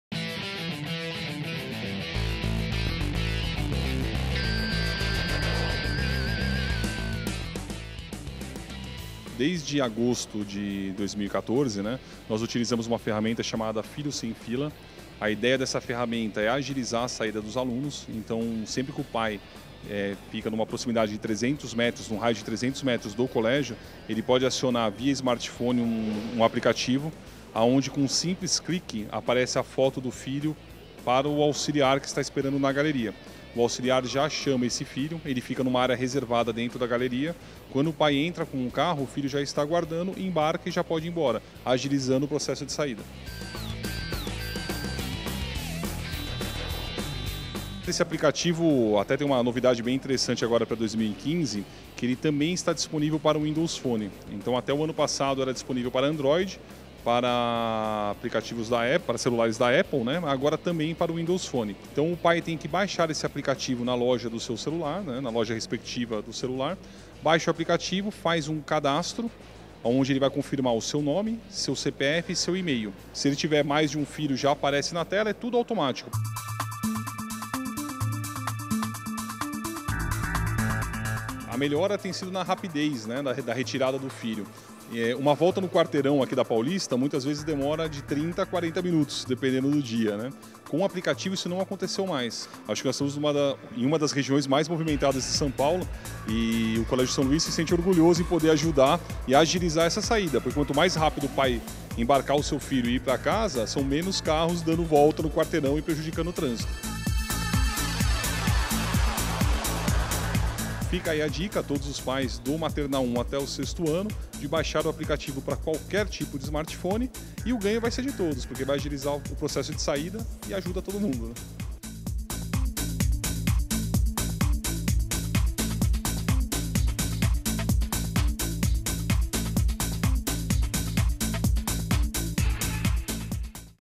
Aplicativo Filhos sem Fila 2015
Saiba como funciona e aproveite de mais essa inovação do Colégio São Luís.